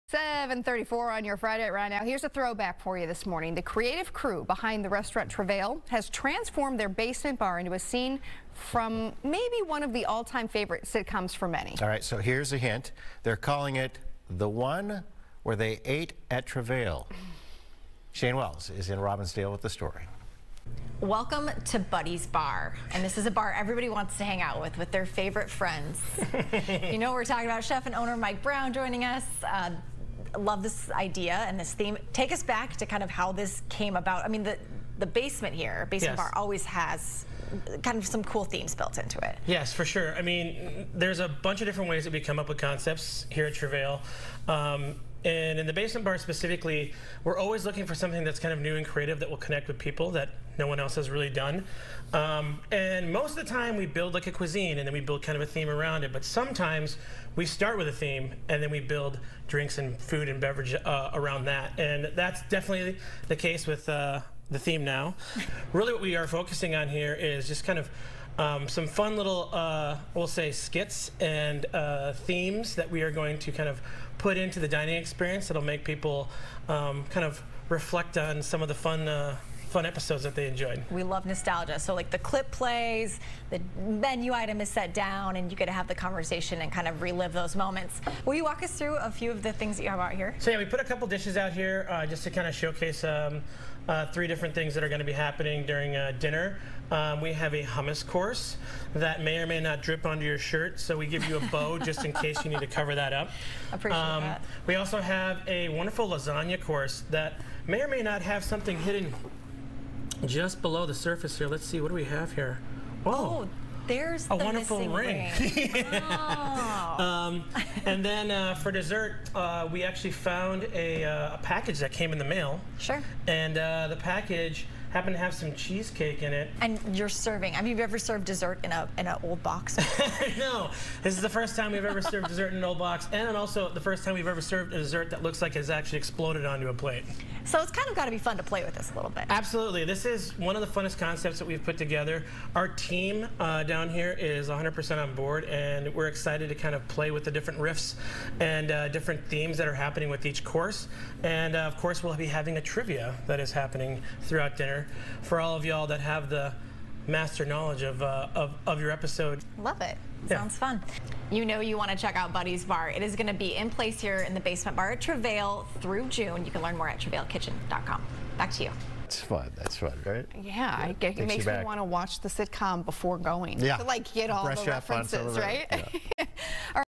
Friends themed dining experience opens at Travail
The sitcom "Friends" first aired 30 years ago and the folks at Travail Kitchen & Amusements were feeling nostalgic, so they’re bringing their love of "Friends" to the Basement Bar. The space has been transformed into an ode to memorable moments and locations in the show. Even the menu was created utilizing real moments from the sitcom. They’ve named it Buddies Bar and if they had to name the episode, it would definitely be "The One Where they Ate at Travail." The pop-up will run through June. It offers an 8+ course tasting menu that includes an ode to Monica’s famous lasagna and Chandler’s stolen cheesecake.